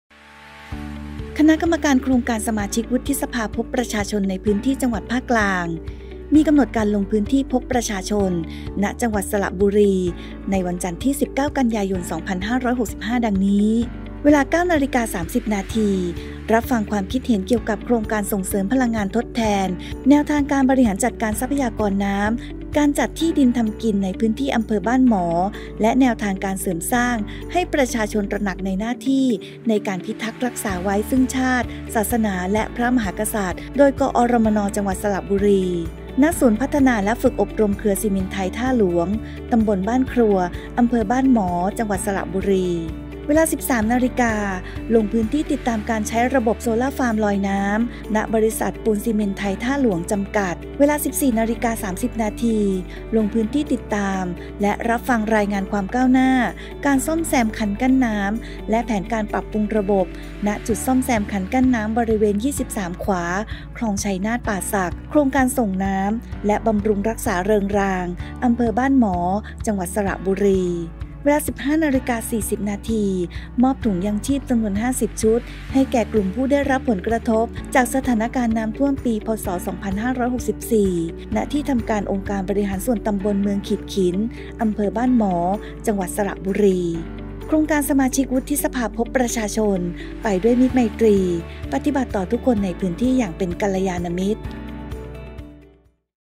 สว.พบประชาชน จังหวัดสระบุรี วันที่ 19 กันยายน 2565
โครงการสมาชิกวุฒิสภาพบประชาชน
“กิจกรรมสมาชิกวุฒิสภาพบประชาชนในพื้นที่จังหวัดภาคกลาง : จังหวัดสระบุรี”
วันจันทร์ที่ ๑๙ กันยายน ๒๕๖๕
ณ ศูนย์พัฒนาและฝึกอบรมเครือซิเมนต์ไทยท่าหลวง ​ตำบลบ้านครัว อำเภอบ้านหมอ จังหวัดสระบุรี